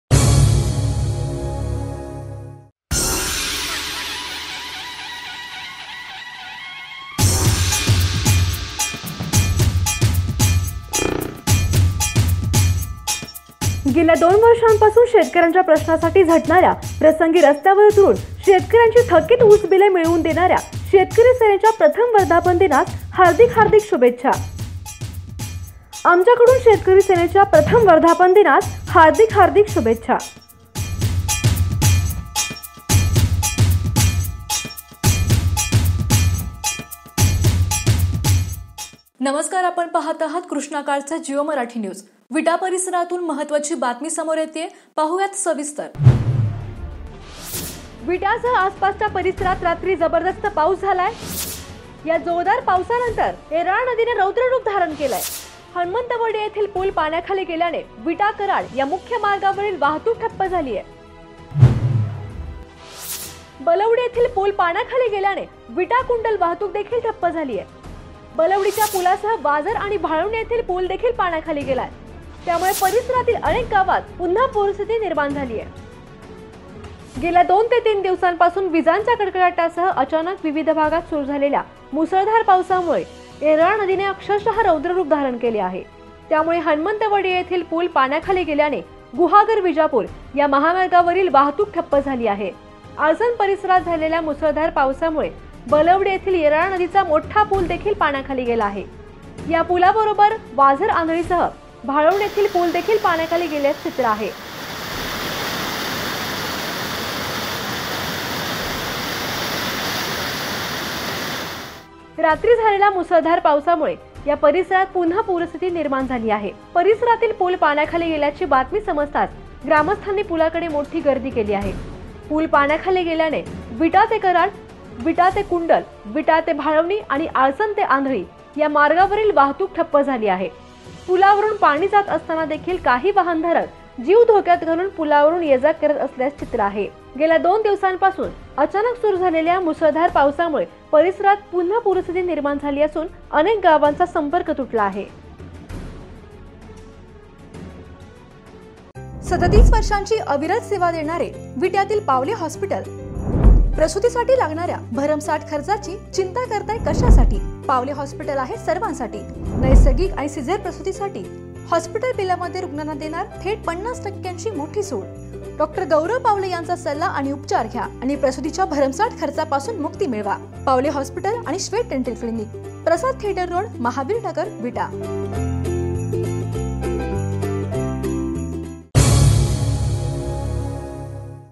■ अतिवृष्टीमुळे विटा परिसरातील अनेक गावांचा संपर्क तुटला
■ विटा परिसरातील अनेक गावातील पूल पाण्याखाली